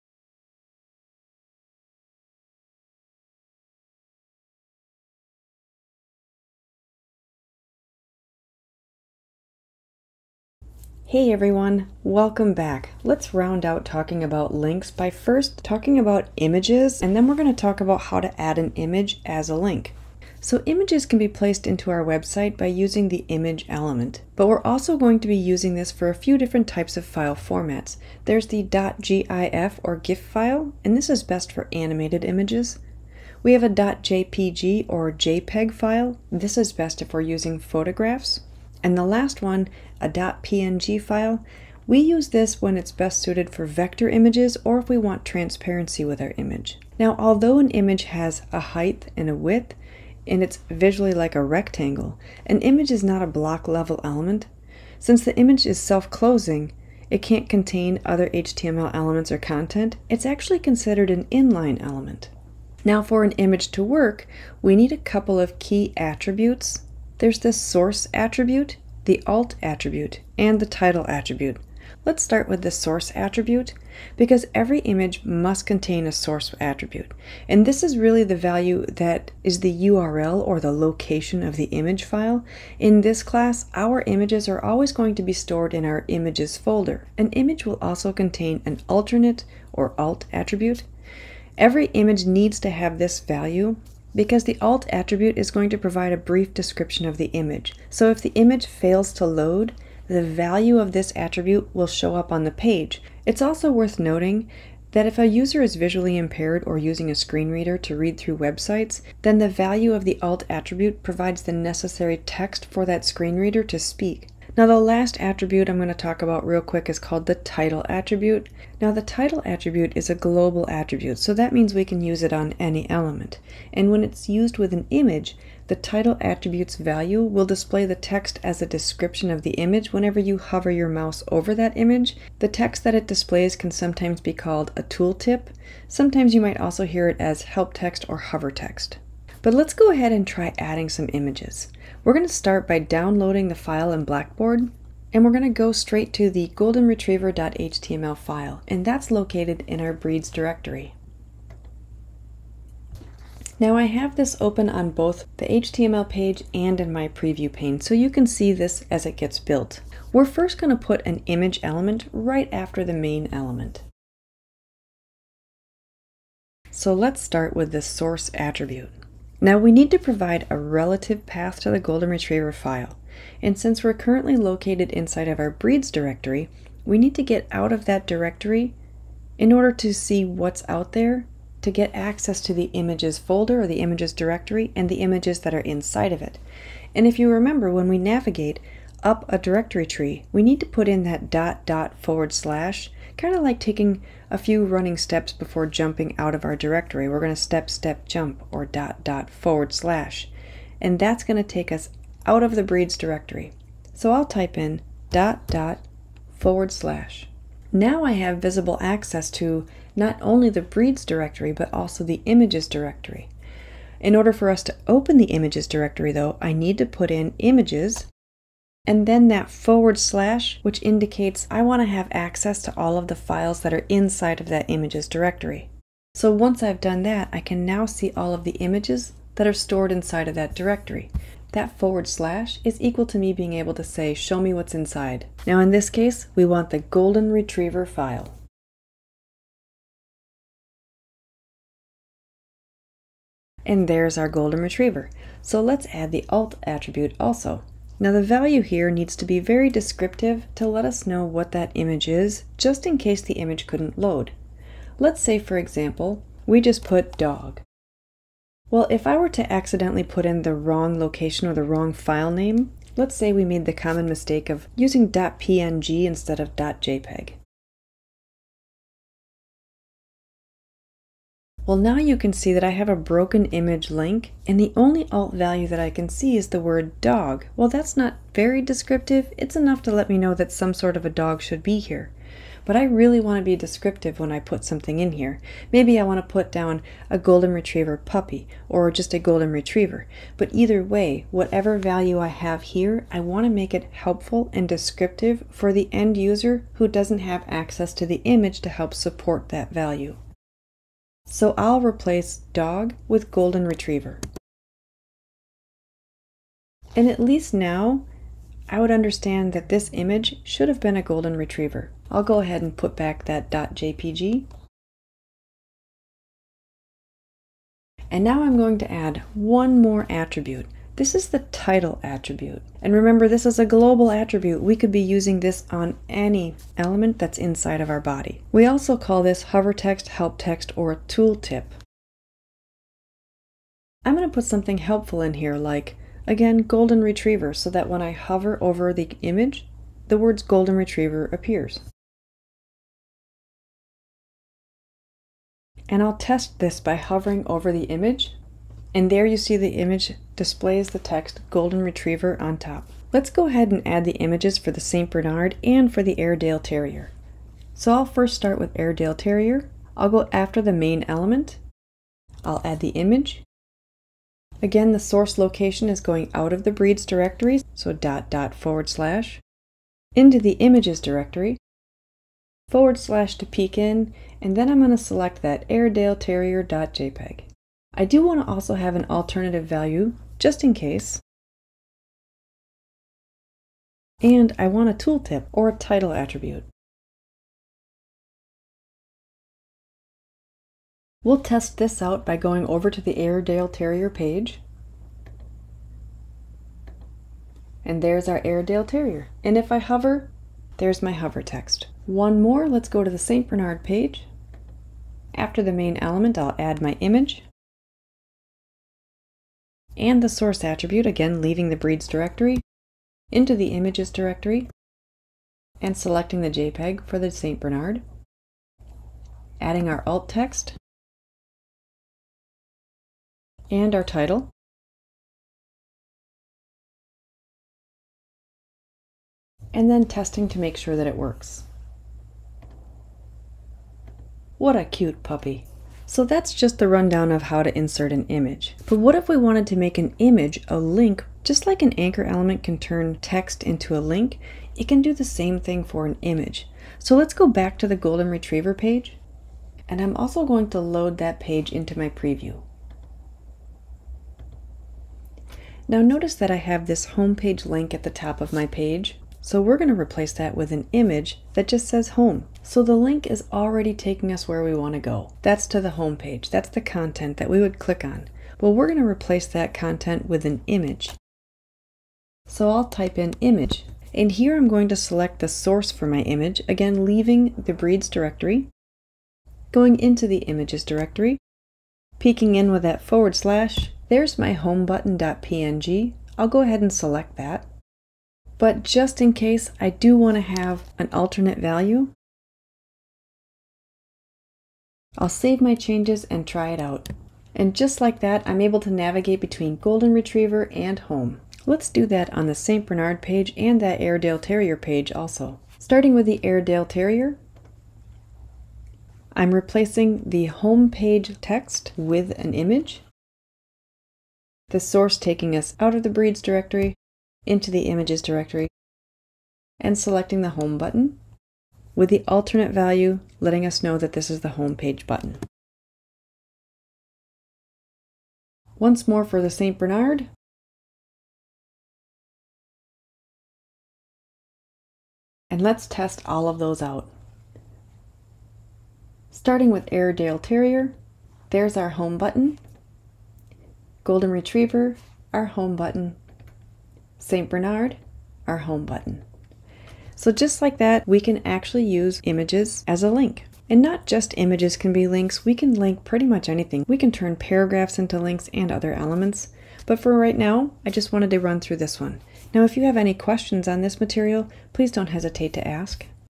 Week 05 Images
Table of Contents:

00:10 - Introduction
00:20 - Image (img) Element
01:02 - 3 Image Attributes:
01:11 - Src Attribute
01:27 - Alt Attribute
01:54 - Title Attribute (tooltip or help text)
07:18 - Images as Links